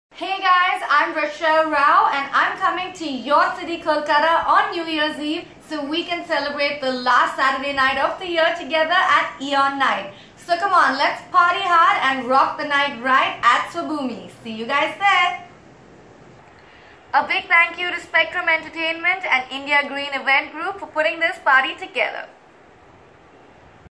ROCHELLE RAO AT SWABHUMI ON EEON NITE 2016
Hey Guys this is Rochelle Rao going live inviting all of you @ SWABHUMI ## Just to Party on the Last Saturday Night of the Year Together at EEON NITE## - Thanks SPECTRUM ENTERTAINMENT & INDIA GREEN EVENT GROUP !!!